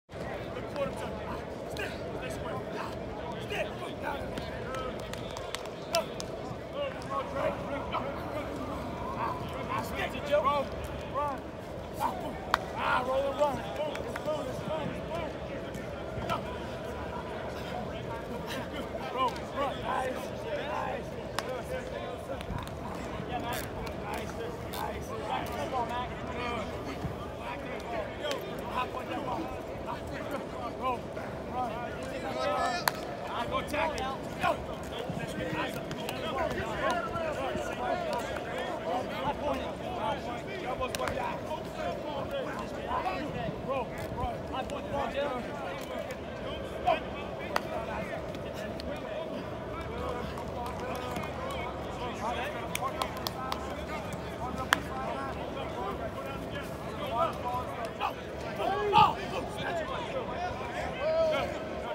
5-star linebacker Ale Kaho makes Alabama practice debut
Ale Kaho (10) at his first Alabama practice after leaving Washington on Aug. 11, 2018 in Bryant-Denny Stadium.

The former five-star recruit who was at Washington left Seattle last week. He had to start the five-practice acclimation period so he was in helmet and shorts without pads. Kaho went through the drills at the end of the line as he learns the Alabama practice routine. His addition was huge considering the lack of depth the Tide has at middle linebacker.